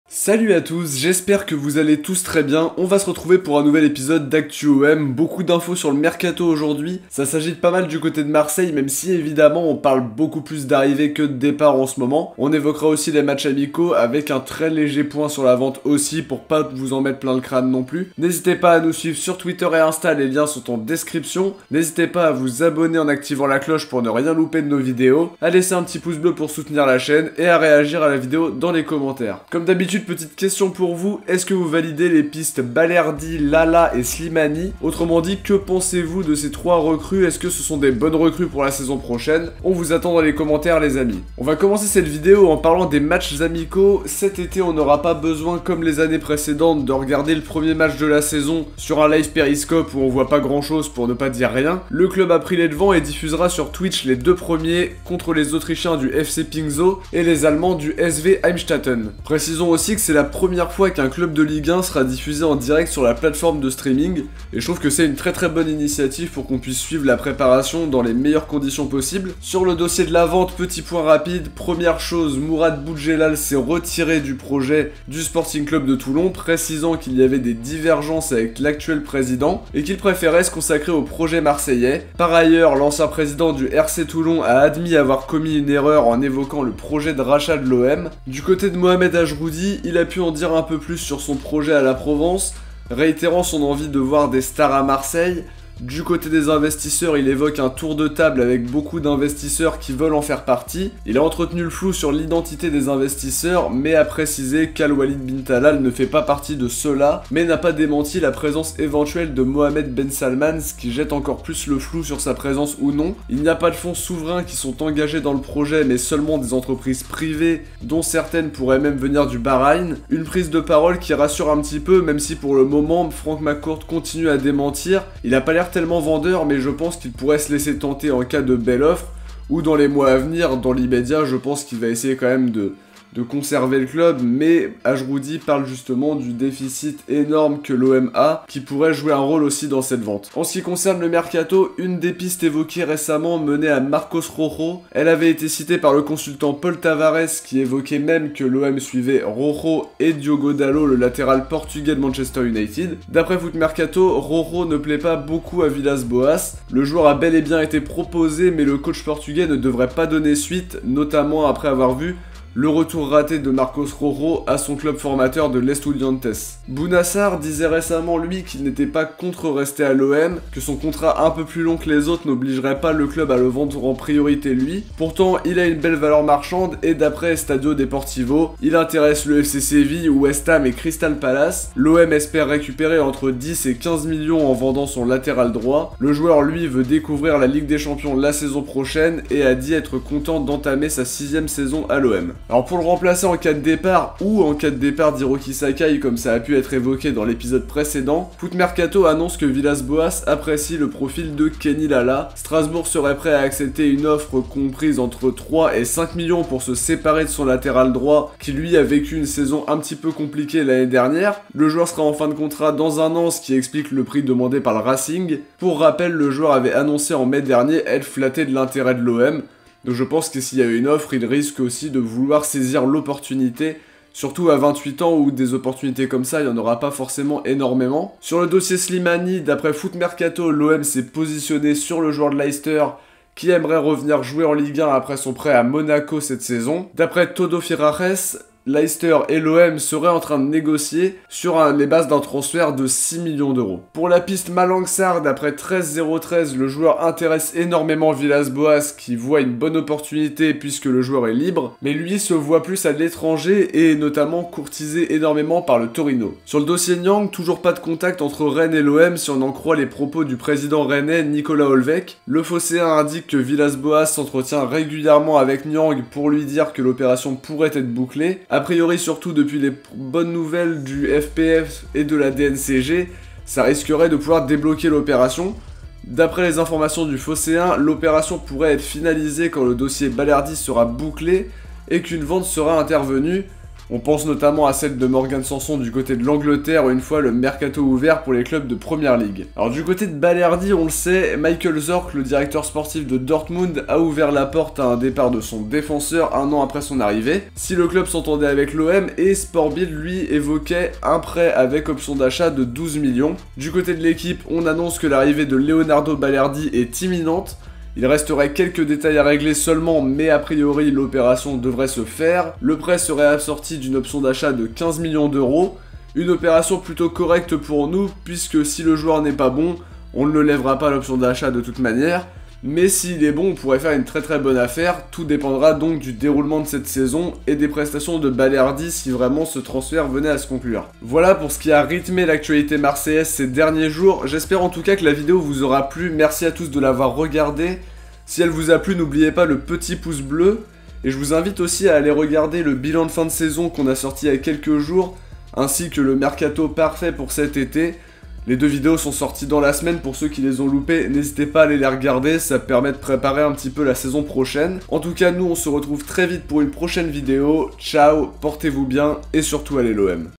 MERCATO OM : SIGNATURE IMMINENTE POUR BALERDI, L'OM SUIT SLIMANI ET LALA, VENTE OM - Actu OM #35
Toutes les infos et news qui font l'actualité de l'Olympique de Marseille dans cette petite série. Au programme :

- Les matchs amicaux diffusés sur Twitch
- Vente OM : Boudjellal lâche le SC Toulon
- Vente OM : Ajroudi précise son projet
- Mercato OM : M. Rojo / D. Dalot suivis ?
- Mercato OM : Bouna Sarr convoité
- Mercato OM : AVB aime Kenny Lala
- Mercato OM : Ça discute pour Islam Slimani
- Mercato OM : Malang Sarr veut l'étranger
- Mercato OM : Mbaye Niang contacté par AVB